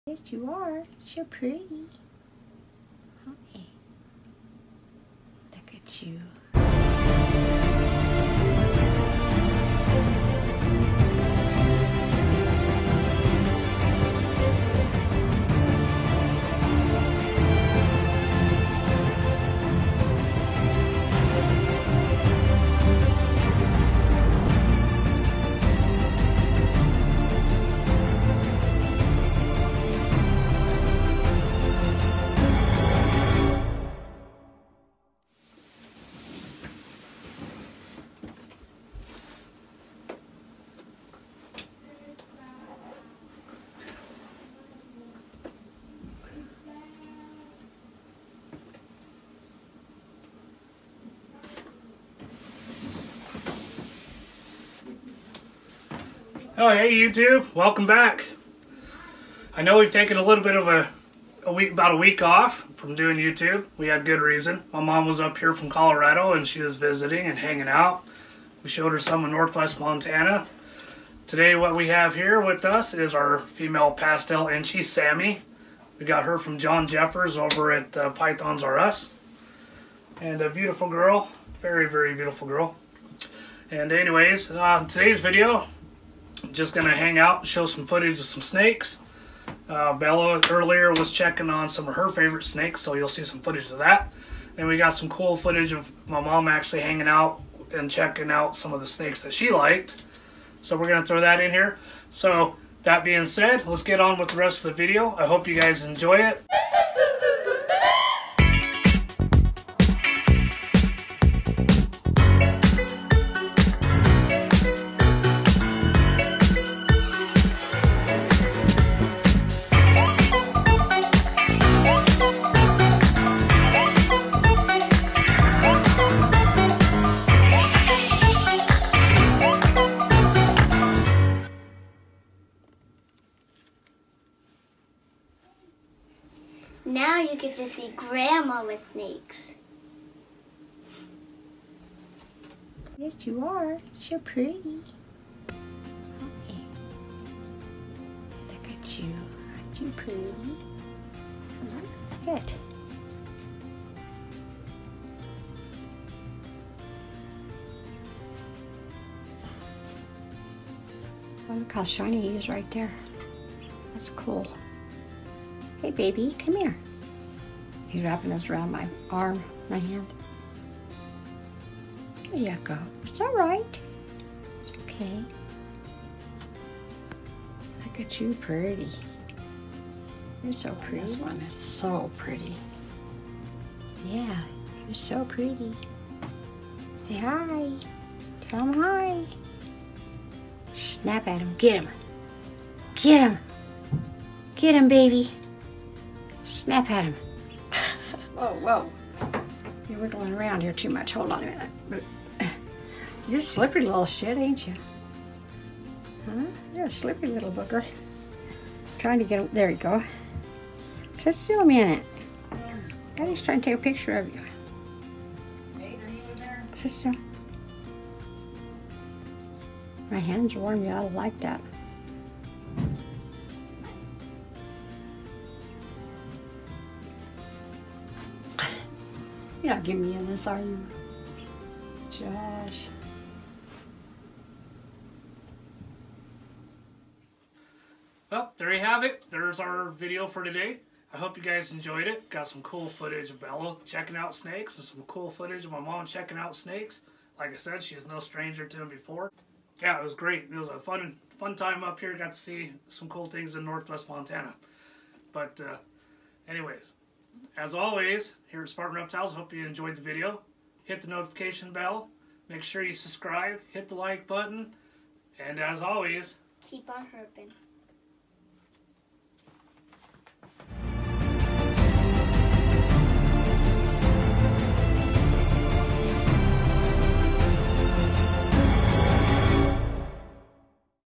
My Mom & Snakes 🙈
Sharing our passions with my mom! She’s no stranger to reptiles. Bella appears in this video with our consent as her parents. This video is not intended for children